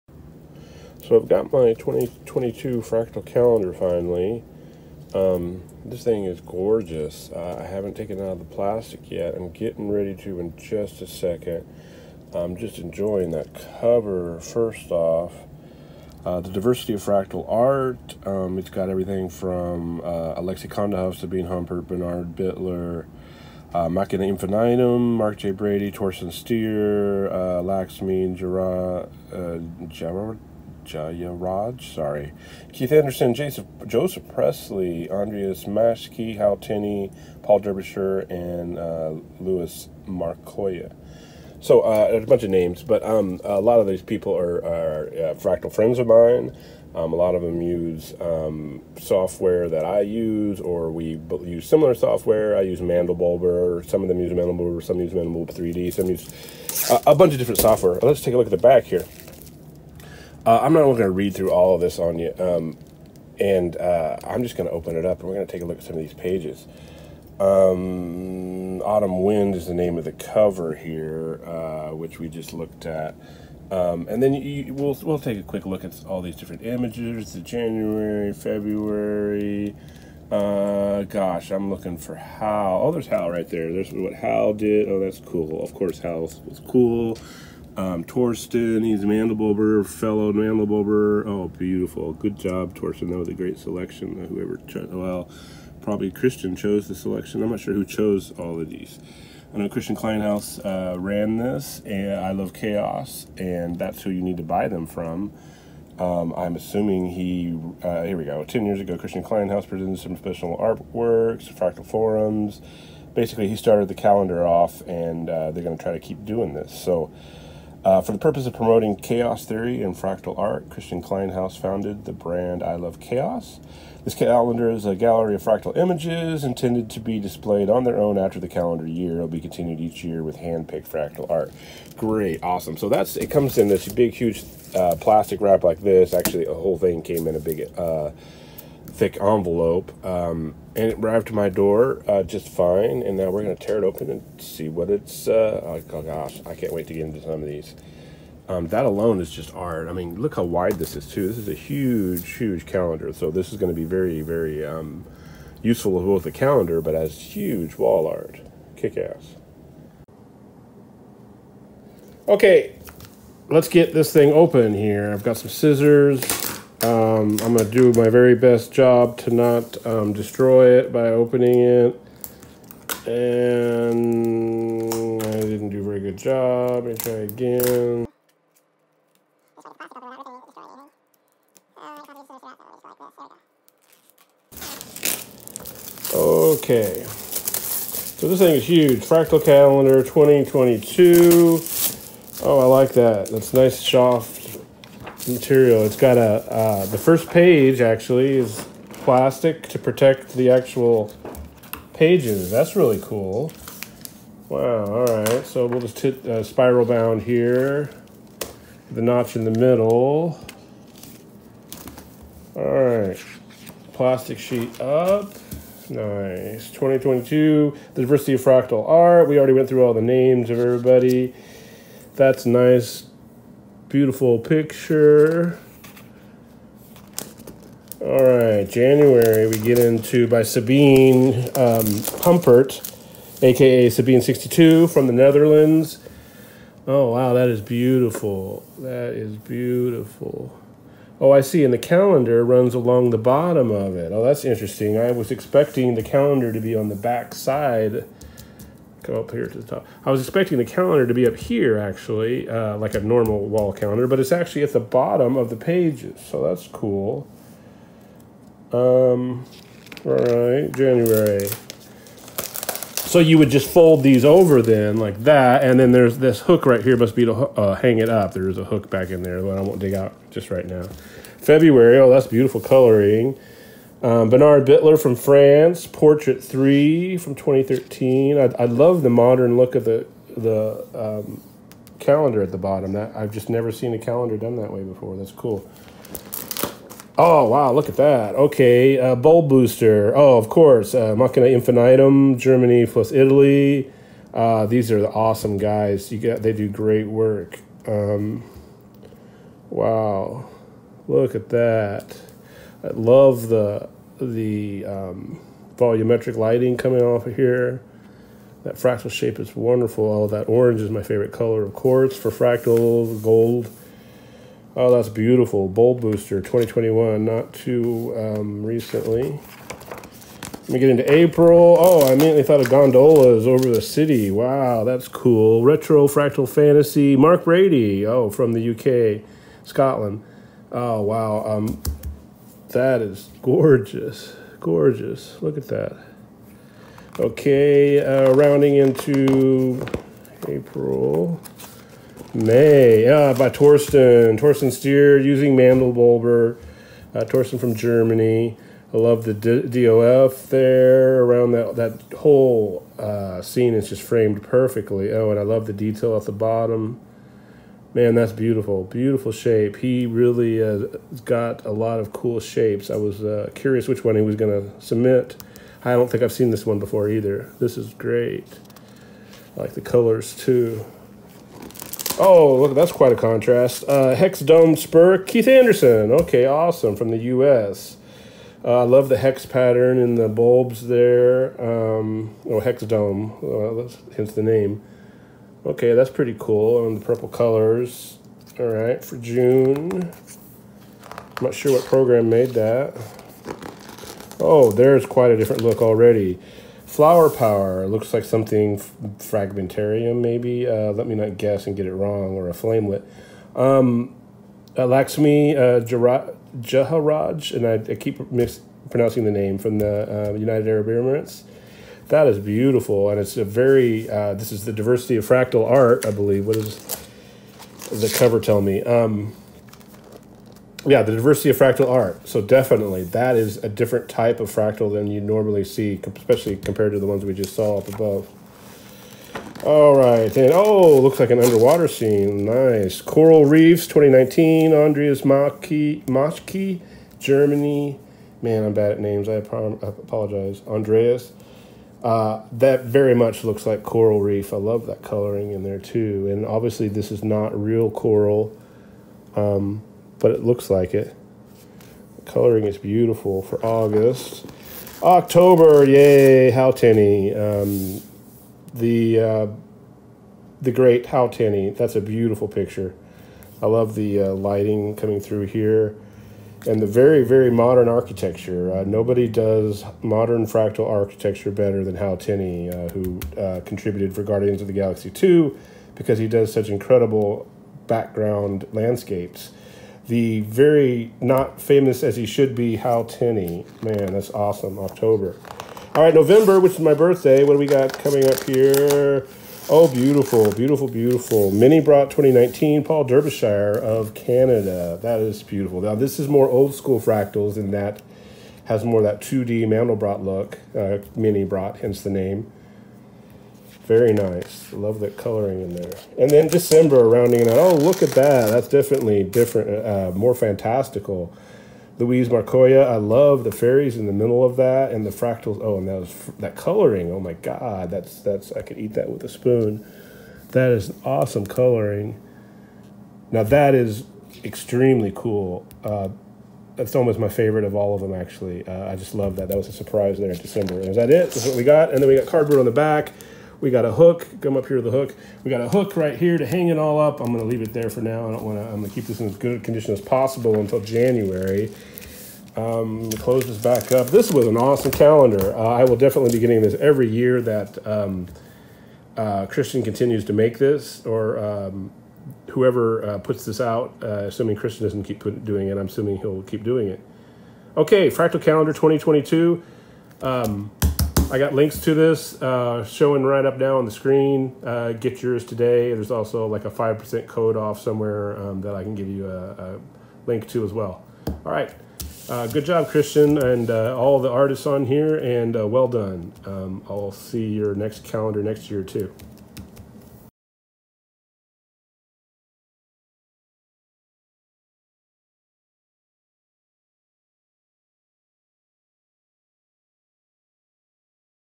Fractal Calendar 2022 Review
I review i love chaos' 2022 Fractal Calendar, featuring A Diversity of Fractals.

Featuring works by Alexey Kondakov, Sabine Humpert, Bernard Bitler, Machina Infinitum (Matteo Scappin & Jesper Nybroe), Mark Brady, Torsten Stier, Keith Anderson, Laxmi Jayaraj, Andreas Maschke, Joseph Presley, Hal Tenny, Paul Derbyshire, and Louis Markoya.